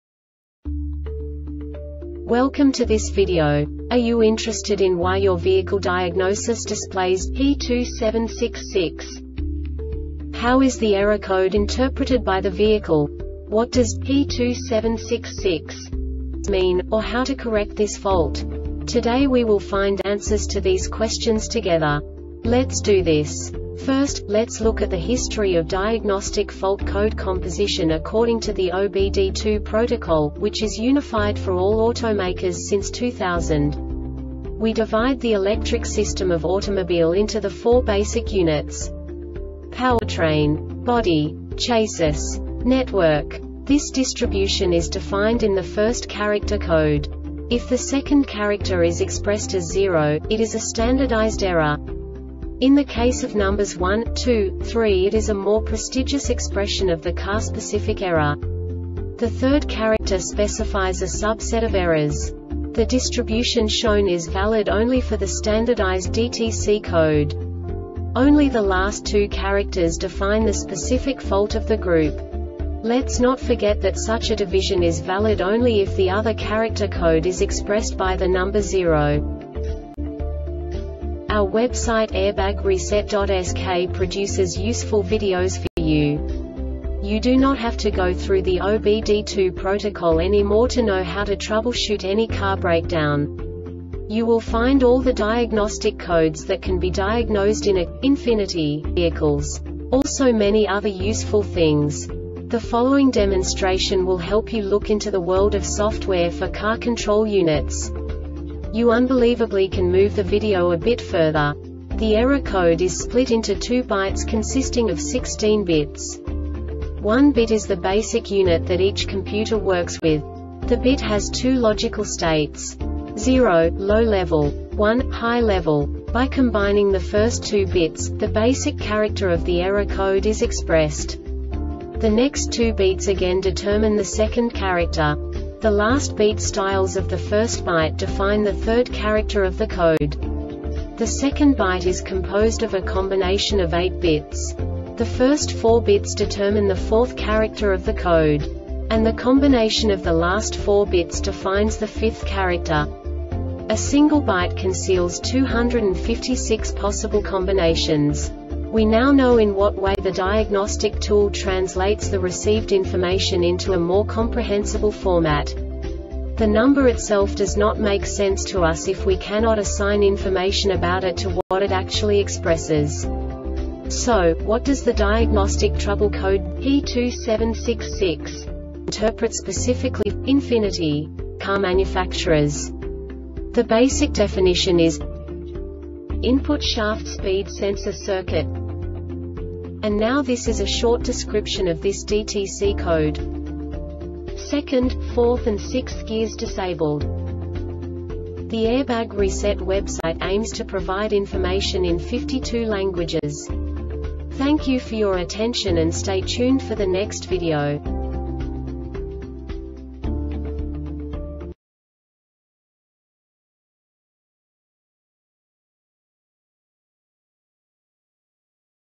DTC Ford P2766 Short Explanation
Contents:
0:21 Basic DTC analysis according to OBD2 protocol standard.
1:48 Insight into programming
2:58 A diagnostically understandable description of the Diagnostic Trouble Code
3:05 Basic error definition

Make:
Ford
Code:
P2766
Definition:
Input Shaft Speed Sensor Circuit
Description:
2nd,4th and 6th gears disabled.